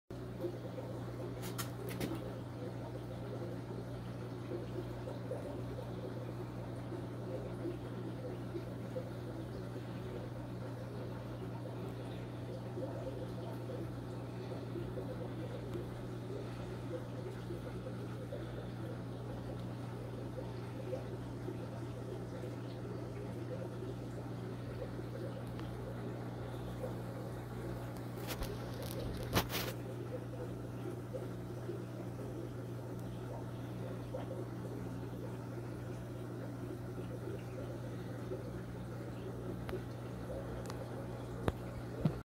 Yellow tailed Congo Tetra swimming (2021)
Yellow tailed Congo Tetra Alestopetersius caudalis swimming around the front of the aquarium, bought from Dan's fish.  Keep Rocking! Thanks!